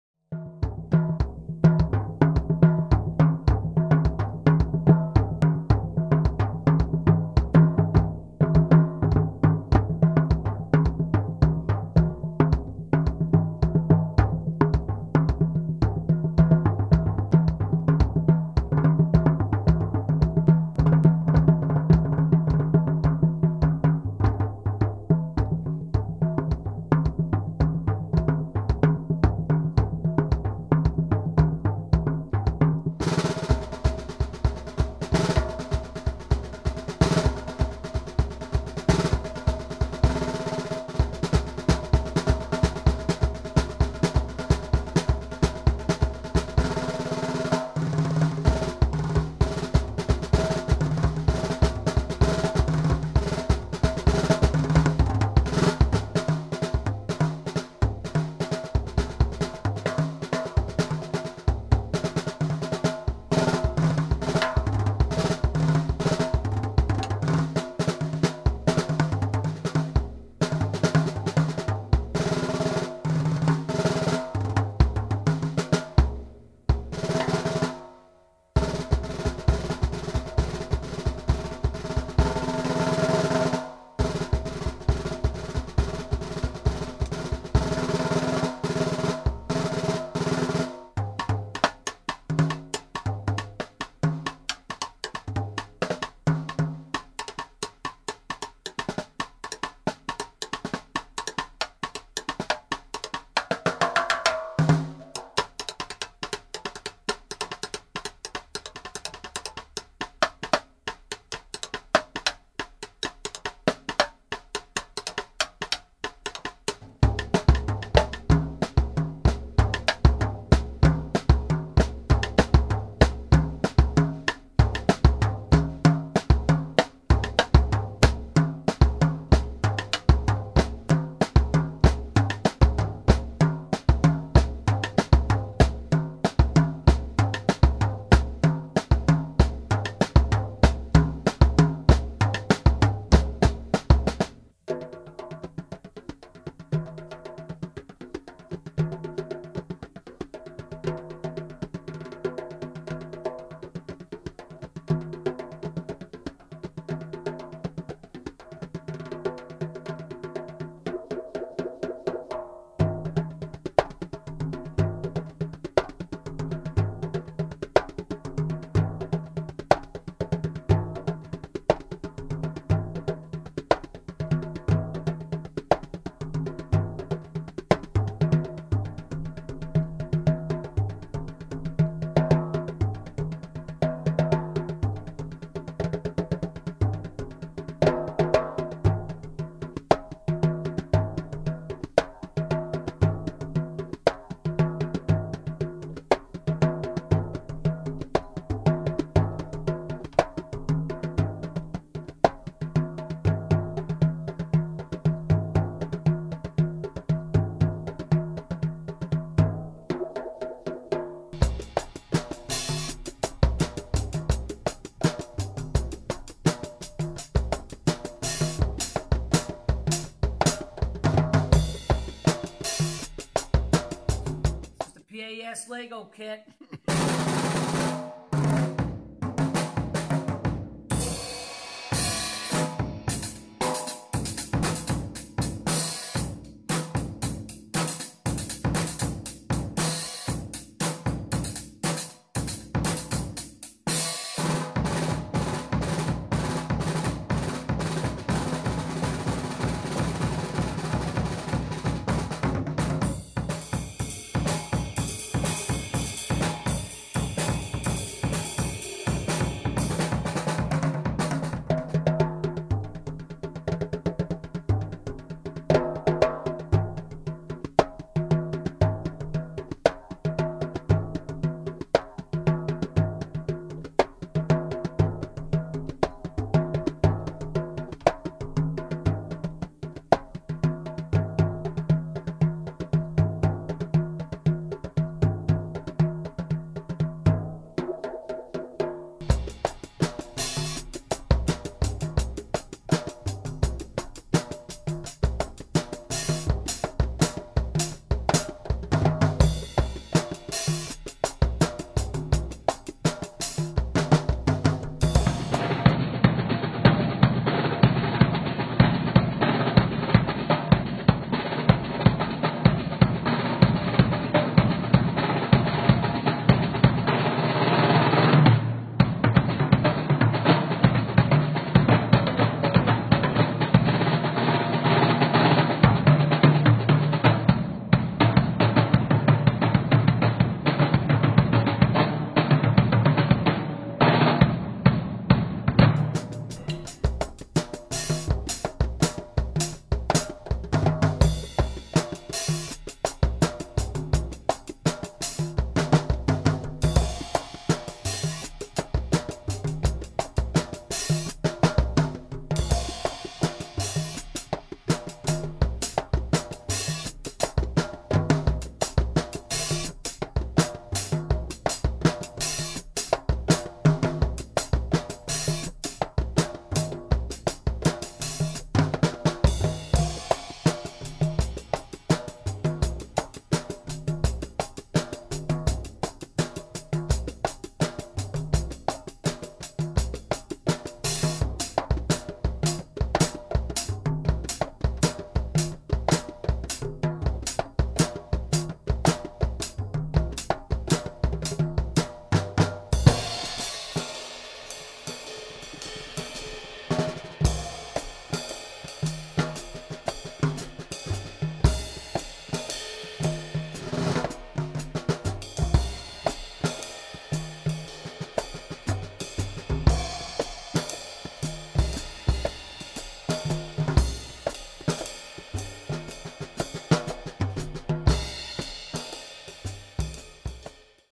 Lego Drum Set for the PAS Rhythm! Discovery Center built & gifted by Brian Melick
My Gift Of A Lego Drum Set to the PAS Rhythm! Discovery Center and Museum.

Honored beyond words to have had my work acknowledged by this global drumming community and to have it in their permanent collection.

Visit PAS Rhythm! Discover Center, 110 W Washington St, Indianapolis, IN 46204

Magical place. With a very dedicated staff who honors and educates visitors about their incredible ongoing and growing collection of percussion instruments both traditional as well as unconventional from all over the world. Both a major hands on discovery center as well as a museum! You won't want to leave!